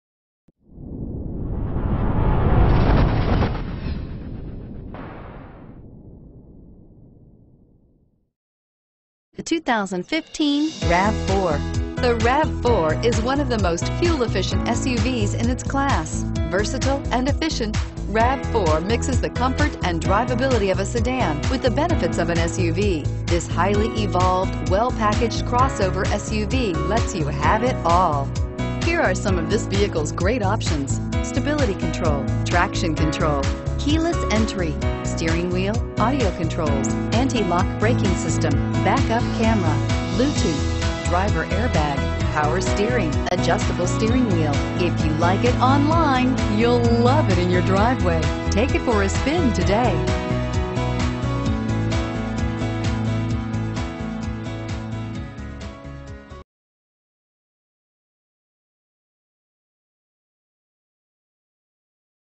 2015 Toyota RAV4 Metairie LA FL3474
2015 Toyota RAV4 LE

Lakeside Toyota
3701 North Causeway

Pick up this Super White 2015 Toyota RAV4, available today at Lakeside Toyota. Featuring Automatic transmission, it has 0 miles. This could be the one you've been searching for.